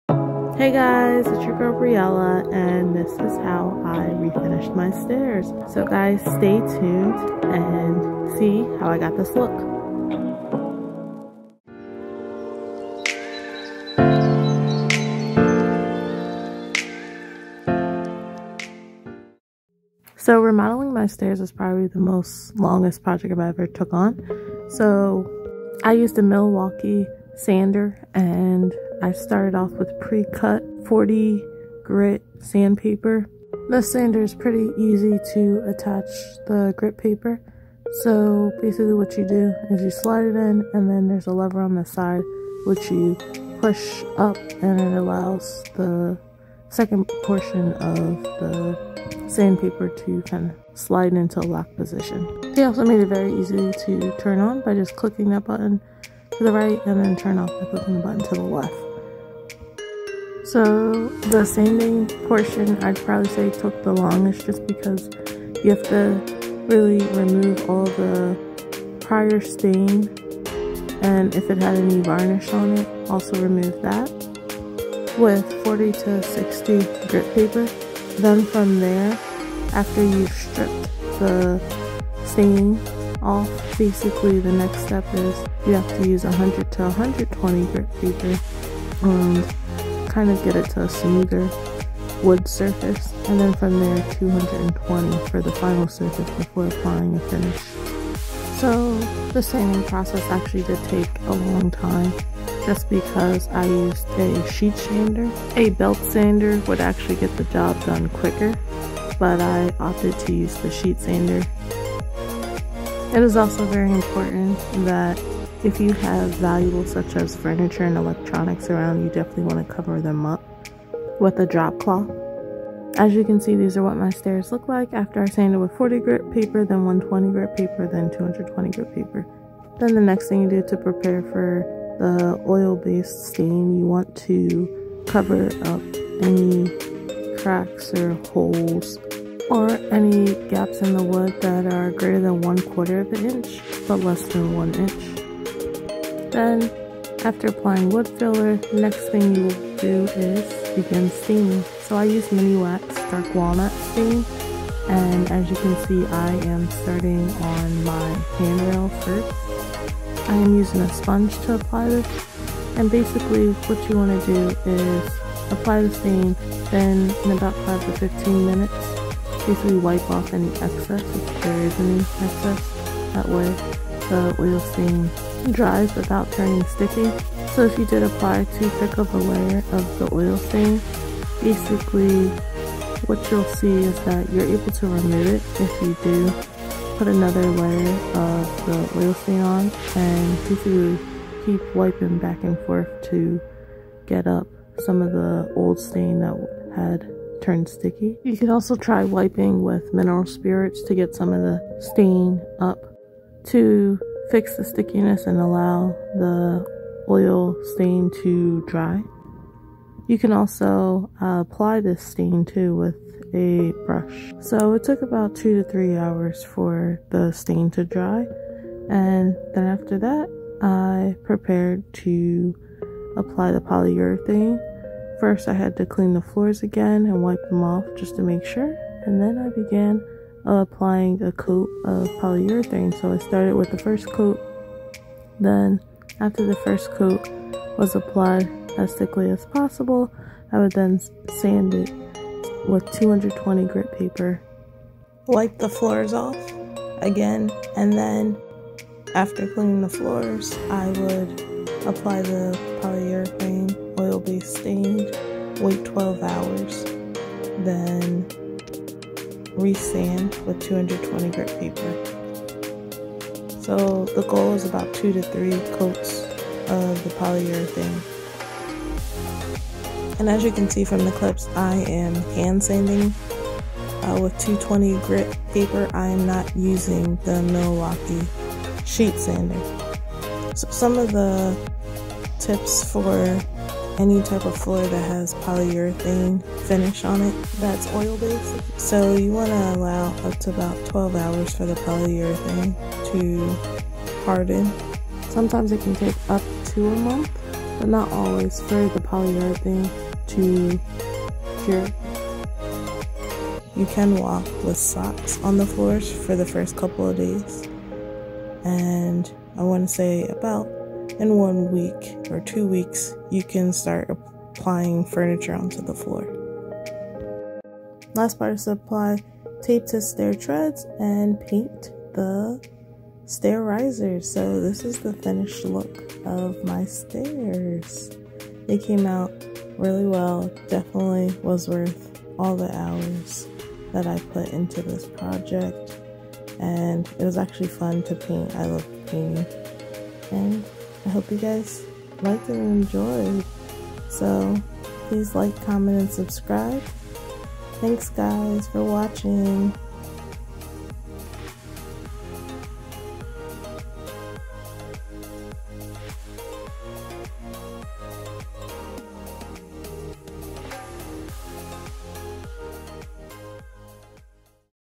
DIY: Re-finish your Stairs for less than 200$; Stair makeover part 2 of 2
Hey Yall and Stella fam!

Today, we will be tackling re-finishing my old stairs. There is a part 1 out already for how to remove carpet off of your stairs

In this part 2 tutorial you will find a How to tutorial for tackling this DIY home project that could save you 1000's of dollars if done by a contractor.

Tools:
Milwaukee palm sander
40 grit paper
120 grit paper
220 grit paper
Corner cat ( I used ryobi)
Paint- Behr paint and primer white semi gloss
Gloves
N95
Vacuum cleaner
Paper towel
Sponge
Soap and water
Miniwax oil stain - dark walnut
Varathrane polyurethrane oil based- high traffic, semi gloss

Musician: Carl Storm

Musician: @iksonmusic